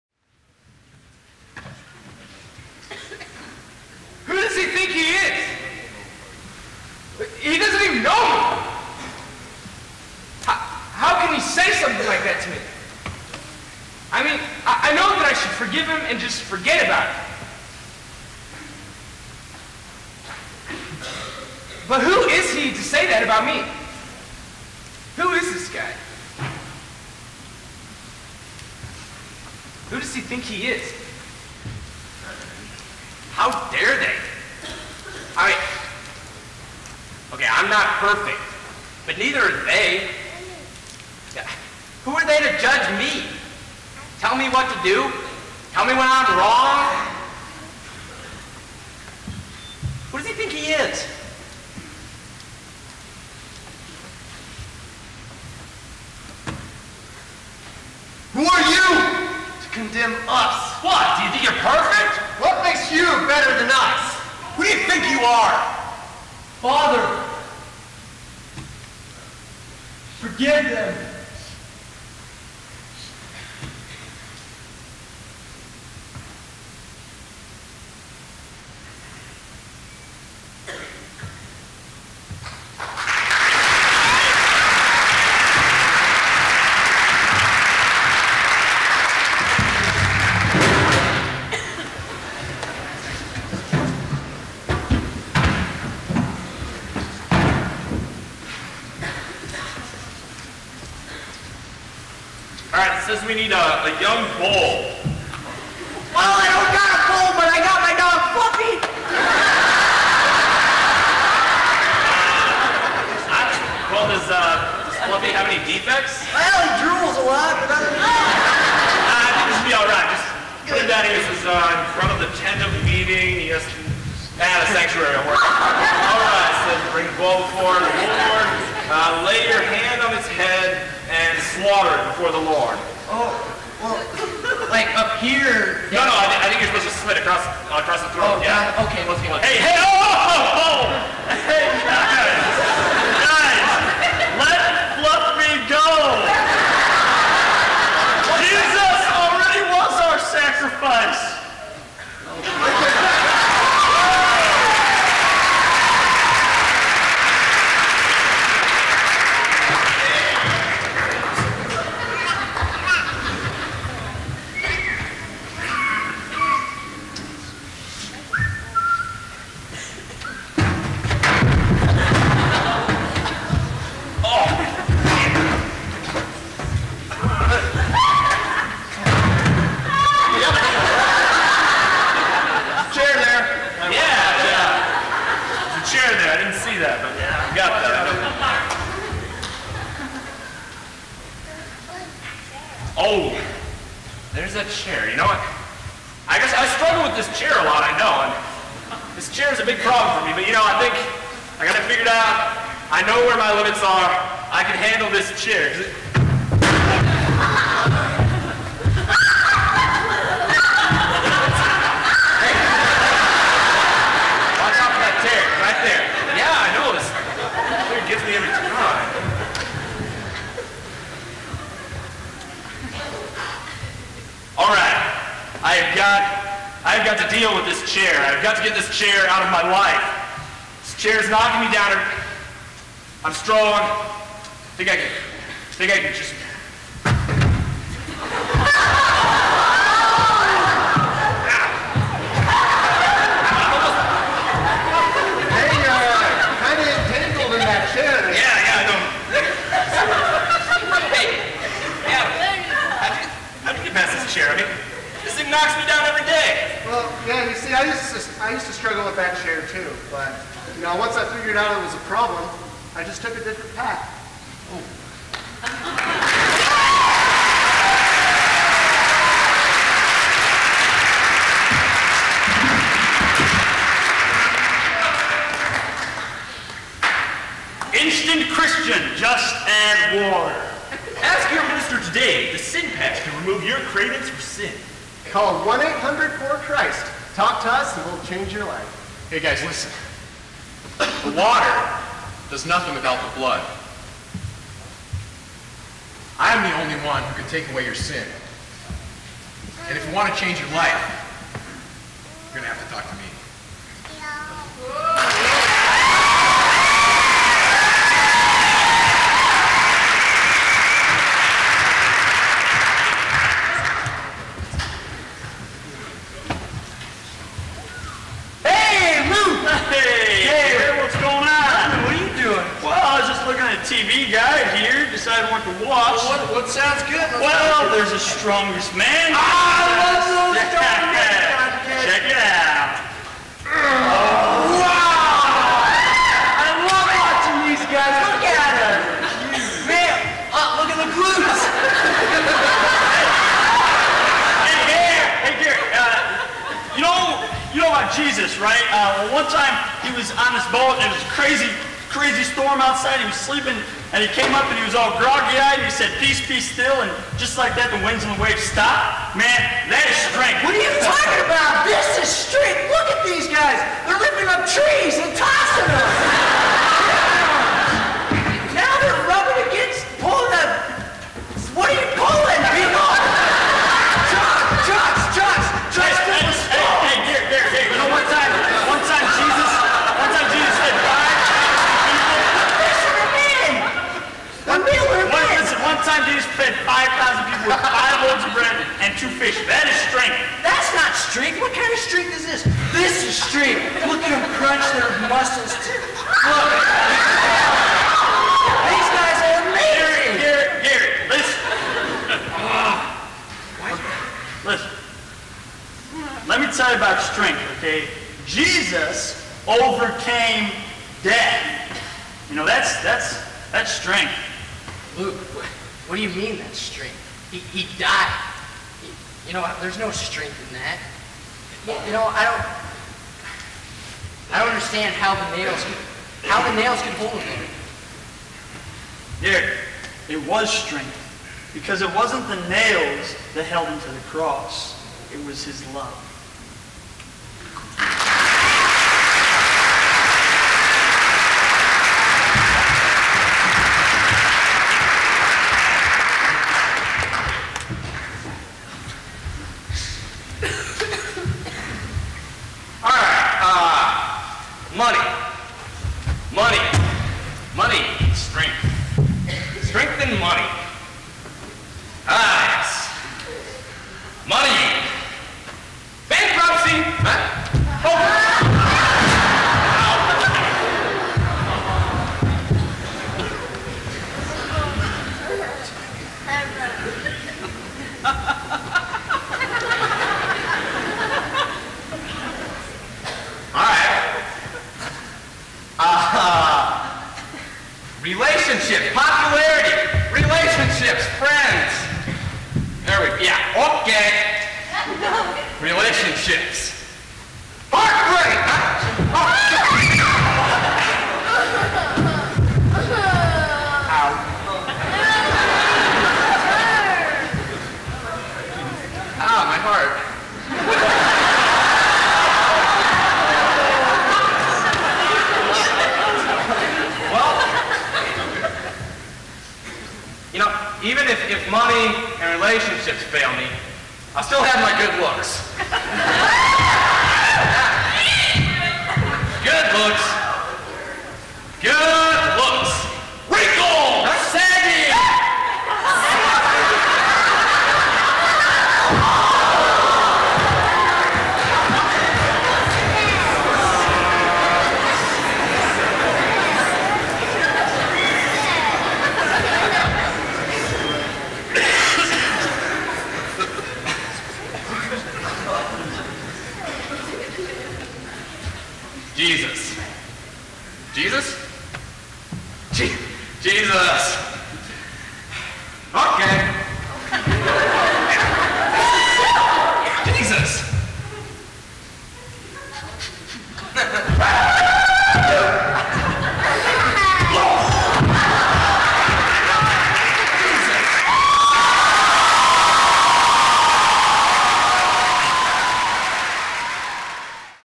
Soul Concern Live! Part 6
This is Part Six in a seven part series of the 2003 Soul Concern DVD.
Soul Concern is a skit group from York College which teaches lessons about Christianity through folding chairs
(skits included)
-Who Does He Think He Is?
-Sacrifice
-Chair There...
-Commercials
-THAT'S Strength!
-Firm Foundation?

I didn't make this video, I'm just putting this up for the enjoyment of all! Sorry about the audio quality, between the crowd, them yelling (or talking softly) and the INTERNET...there's only so much one can do! I hope you like it regardless, because there's a message in every skit I hope you don't miss.